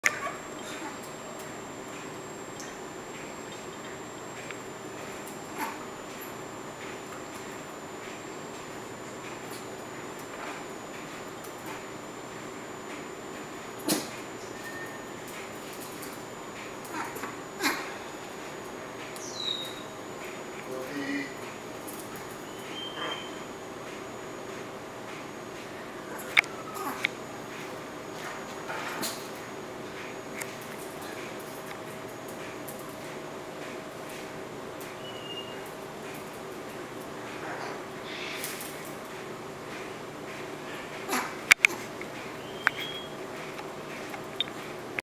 African Gray vs Chihuahua
My African Gray having a showdown with my chihuahua. The gray has her wing clipped because her dad managed to open his cage and fly away, it was done to prevent her from escaping.